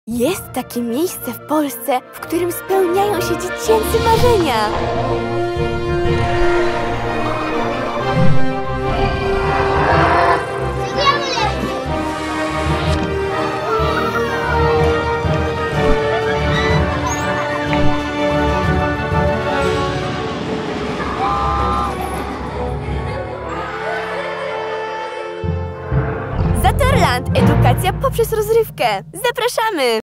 Zatorland - Edukacja poprzez rozrywkę!
Jest takie miejsce w Polsce, w którym spełniają się dziecięce marzenia!
Spot reklamowy stworzony dla Parku Rozrywki Zatorland w Zatorze.